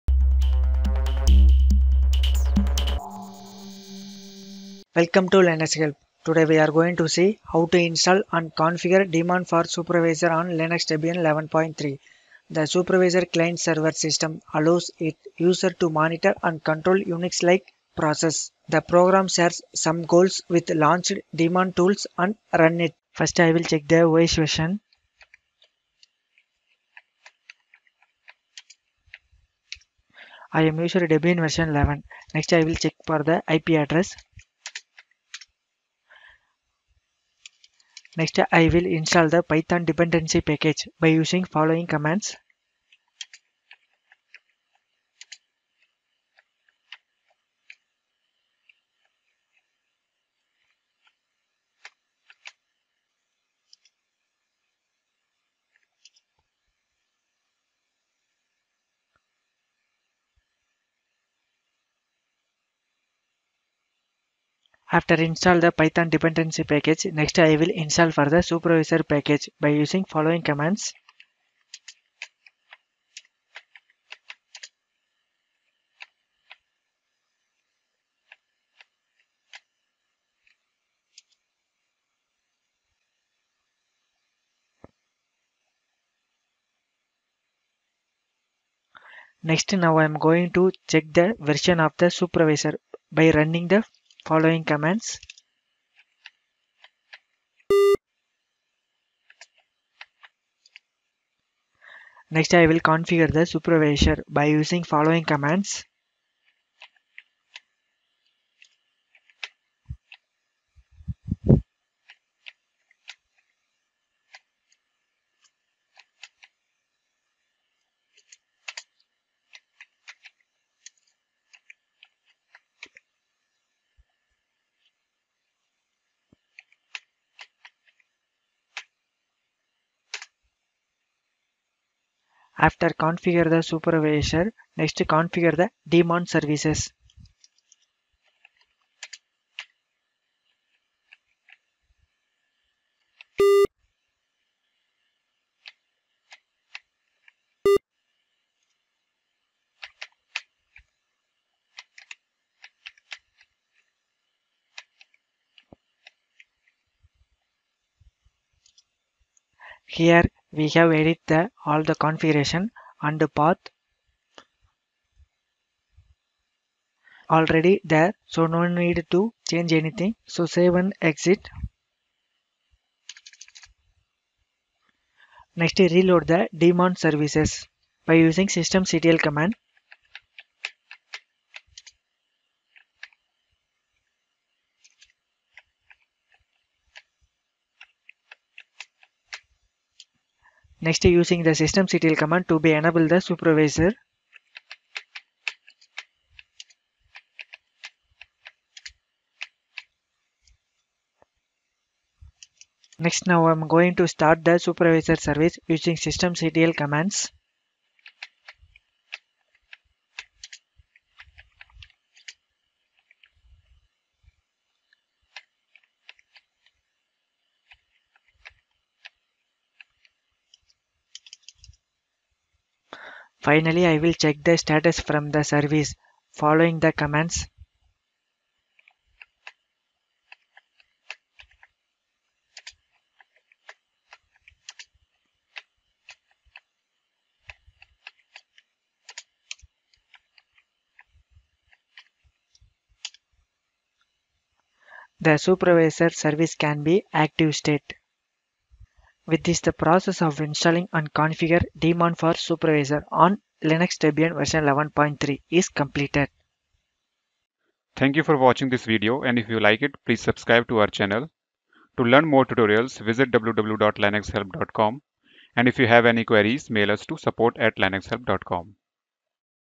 How to install and configure daemon for supervisor on Linux Debian 11
The Supervisor client/server system allows its users to monitor and control UNIX-like processes. The program shares some goals with launchd, daemon tools, and runit.